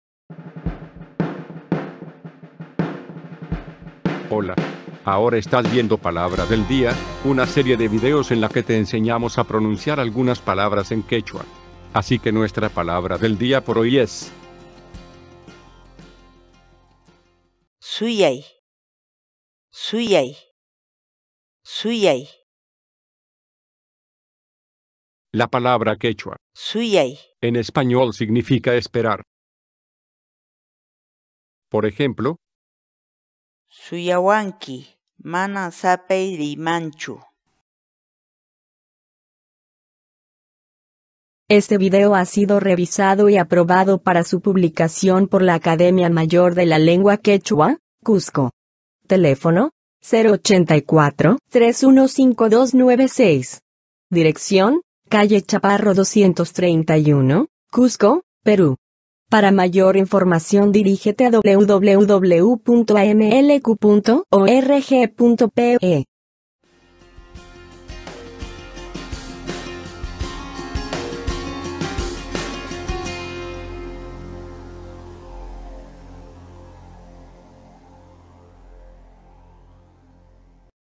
Como pronunciar suyay ‘esperar’
Social

Grupo de Investigación Hinantin: La entrada ‘suyay’ es parte de la base para el desarrollo de un diccionario hablado disponible gratuitamente para el idioma quechua.